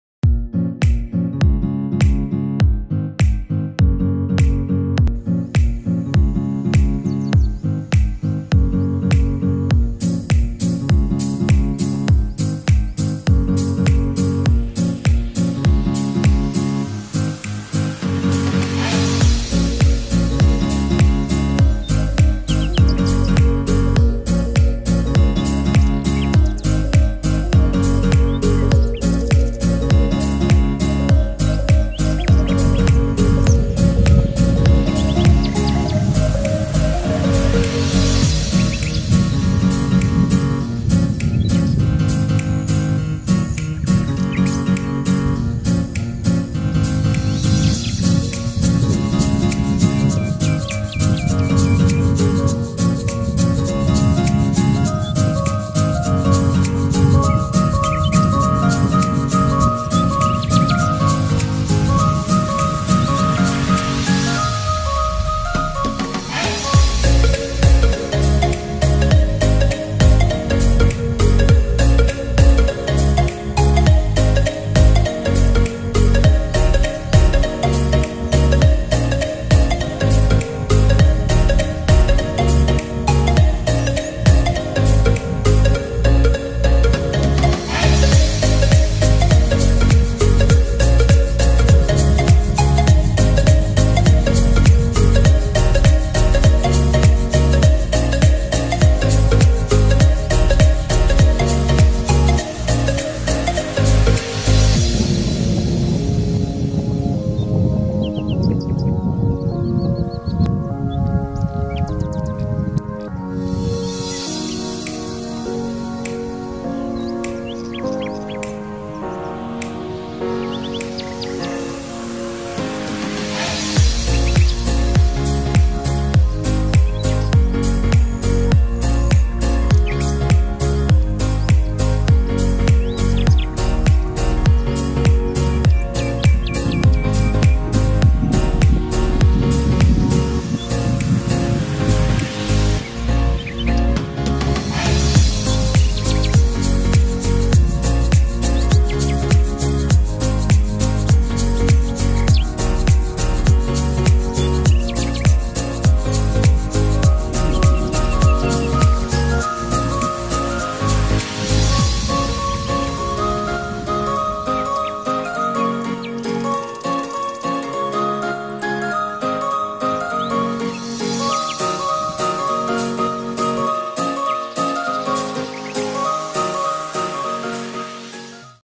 Barn Swallow (Hirundo Rustica)
Barn Swallow are migratory birds. Breeds in Pakistan hills, Himalayas and North East India; widespread farther south in winter.

This video is taken on Hoskote lake on 25thFeb 2018.